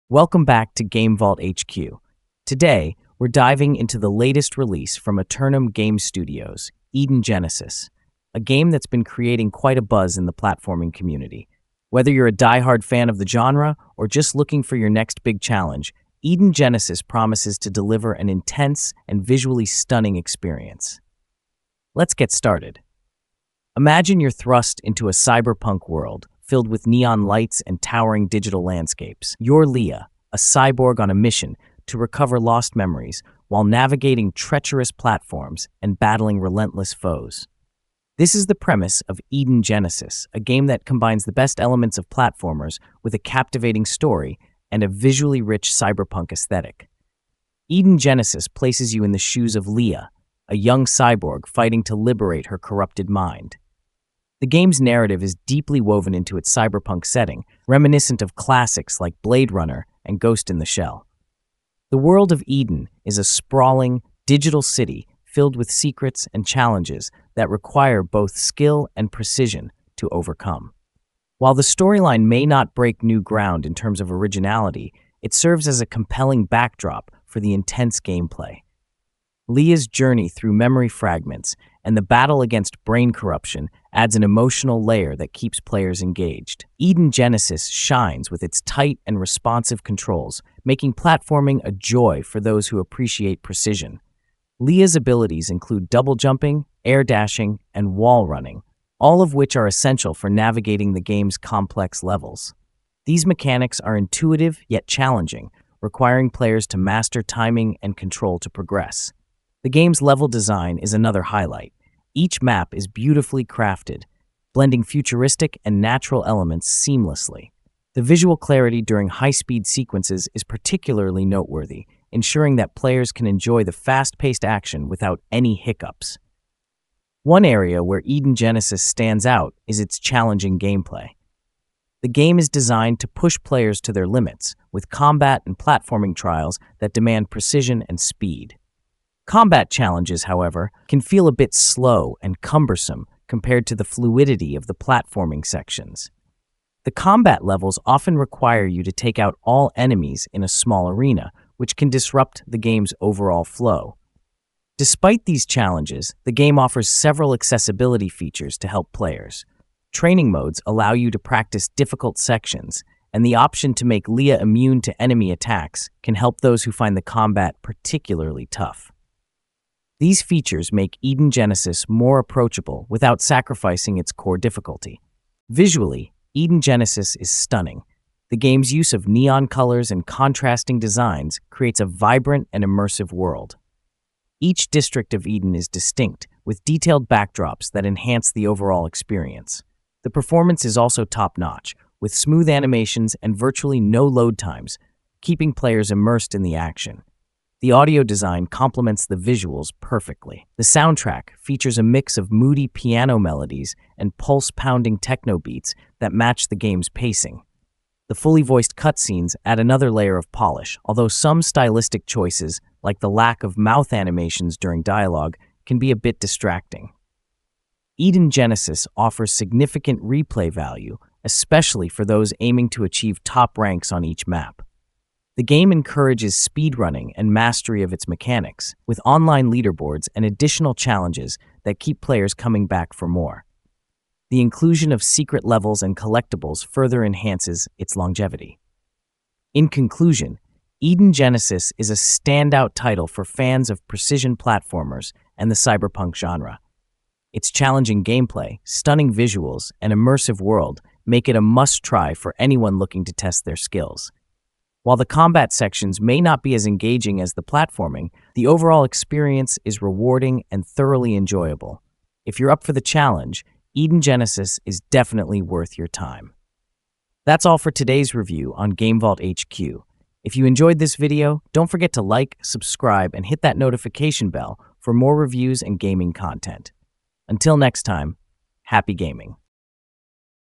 Eden Genesis Review: The Ultimate Cyberpunk Challenge!
Welcome to GameVault HQ! In today's video, we bring you an in-depth review of Eden Genesis, the latest cyberpunk platformer from Aeternum Game Studios. We'll cover everything from Eden Genesis gameplay and mechanics to our detailed Eden Genesis impressions. If you're into challenging games or precision platformers, this one's for you!

In Eden Genesis, you'll explore a stunning cyberpunk world on platforms like Xbox, PlayStation, PC, and Nintendo Switch. Our Eden Genesis walkthrough will guide you through the game's intricate levels and help you master its demanding mechanics. Released in 2024, this game is quickly becoming a must-play for fans of the genre.

Key points covered in this review:

Overview of the Eden Genesis story and setting
Detailed Eden Genesis gameplay analysis
The challenging yet rewarding platforming mechanics
Visual and audio design breakdown
Our final thoughts on whether Eden Genesis is worth your time

Share your thoughts on Eden Genesis in the comments below. Have you played it? What do you think of its challenging platforming mechanics and cyberpunk aesthetic?